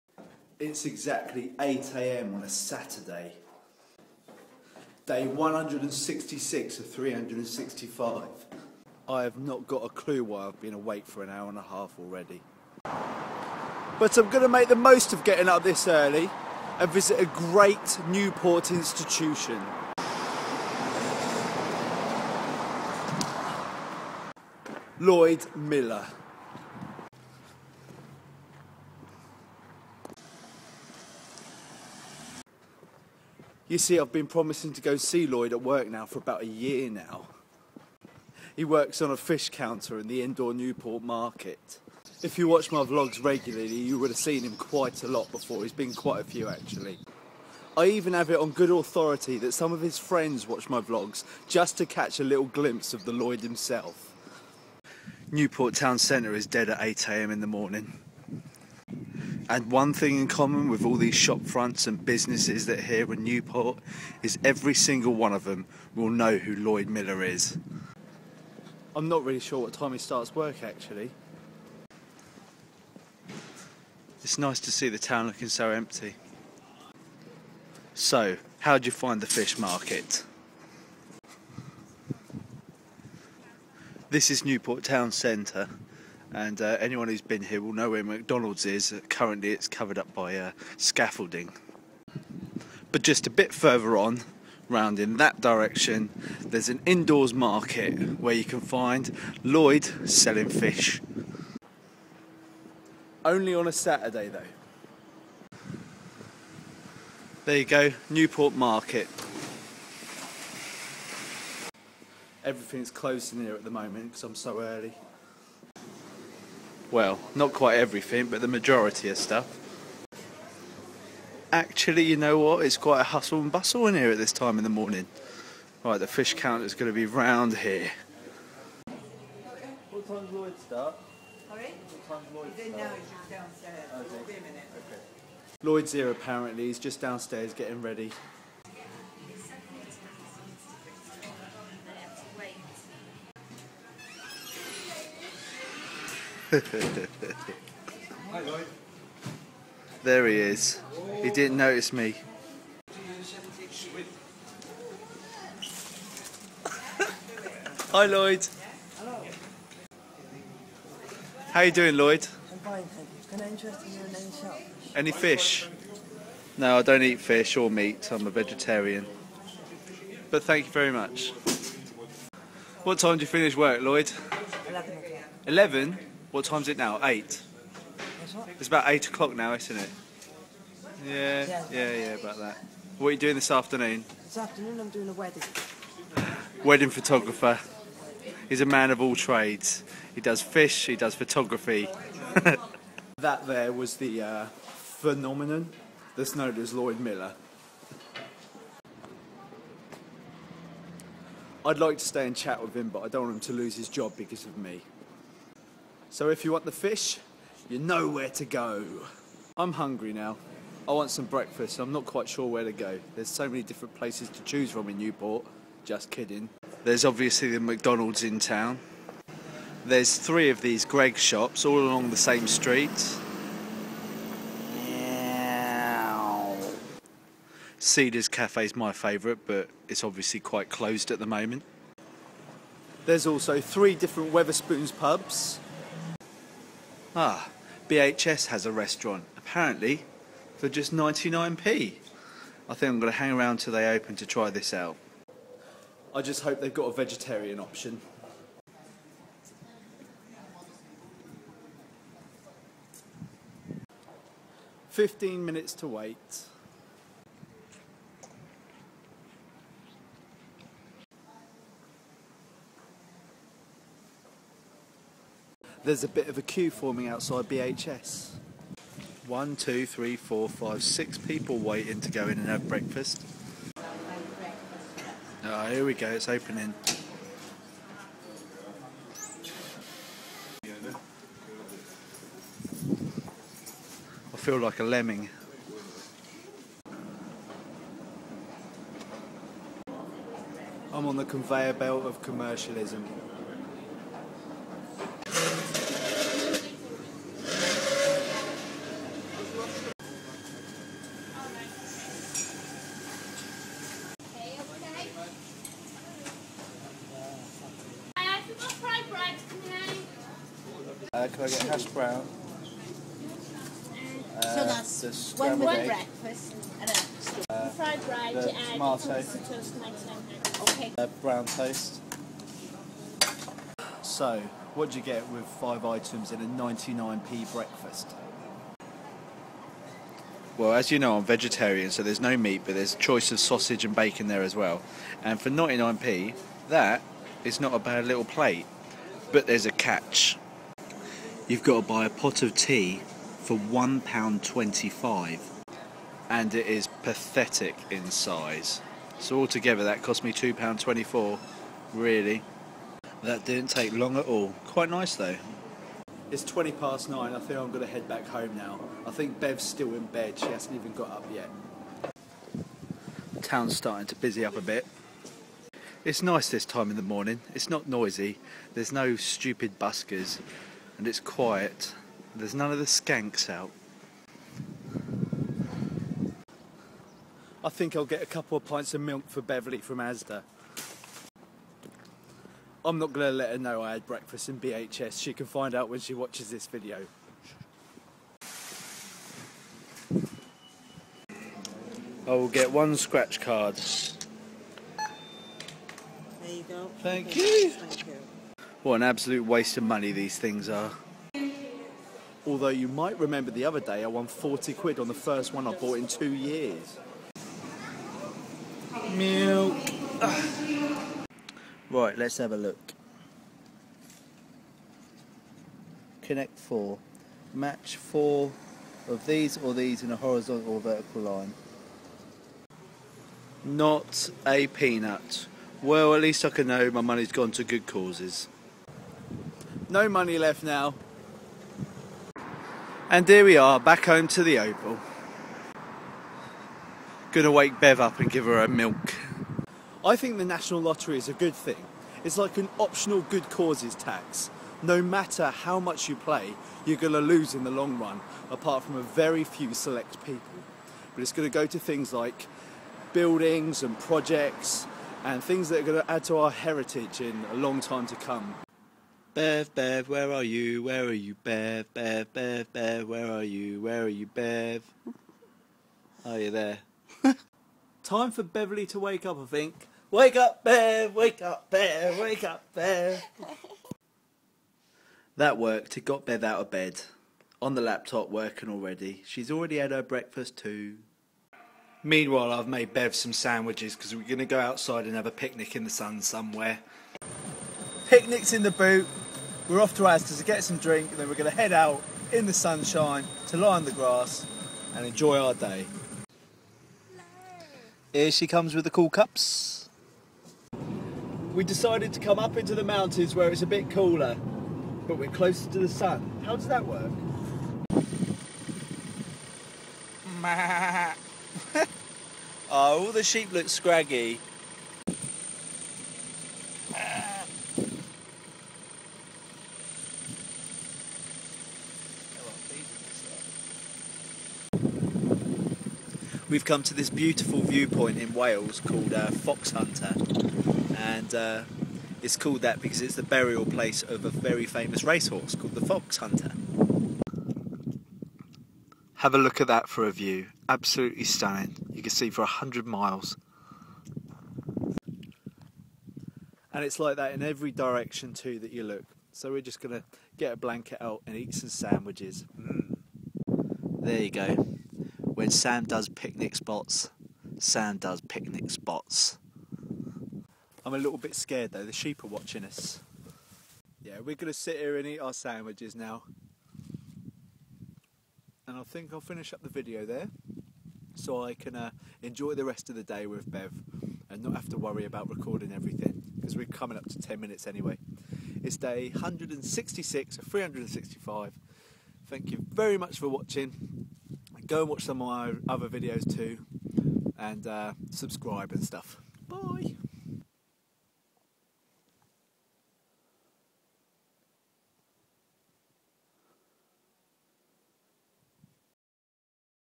166/365 Sam Cornwell meets Lloyd Miller at the fish market
Sam Cornwell wakes up way too early on a Saturday so decides to go and see the infamous Lloyd Miller at work at his fish counter. He then takes Bev Mason on a picnic up a mountain. It's been a lovely day.